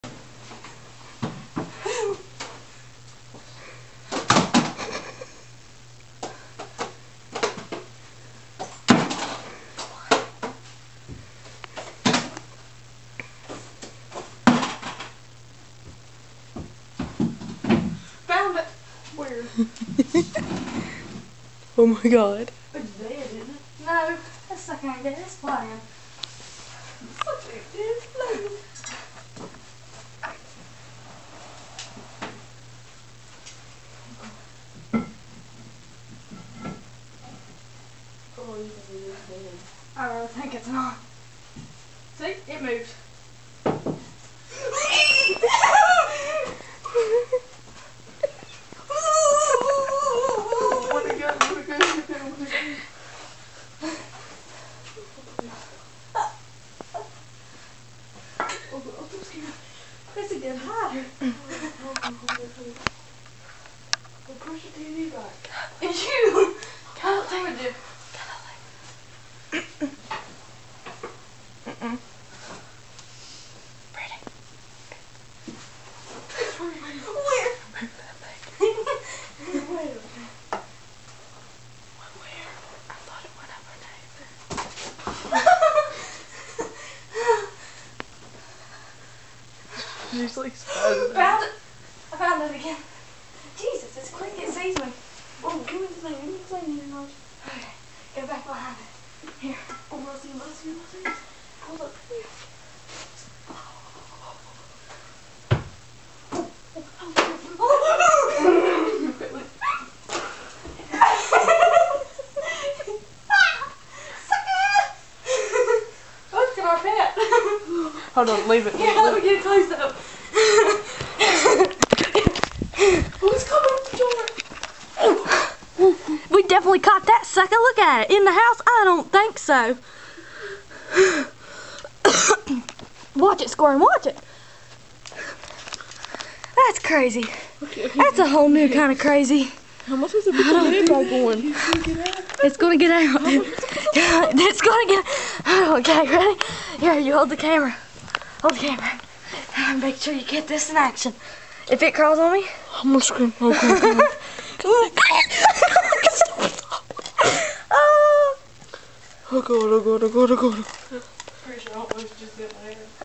Capturing the lizard
lizad the lizard being captured by Kylie and Brittany
(britt white shorts kylie pink shorts)